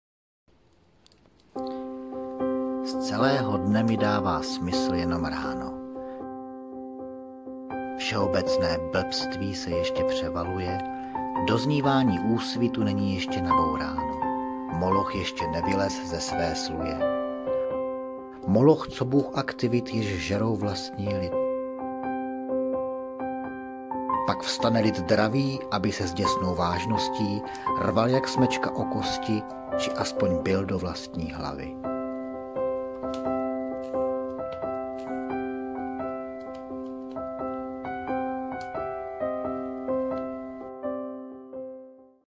Velebení rána...
Z celého dne mi dává smysl jenom ráno
Všeobecné blbství se ještě převaluje
Doznívání úsvitu není ještě nabouráno
Moloch ještě nevylez ze své sluje

Moloch co bůh aktivit
jež žerou vlastní lid

Pak vstane lid dravý
aby se s děsnou vážností
rval jak smečka o kosti
či aspoň bil do vlastní hlavy...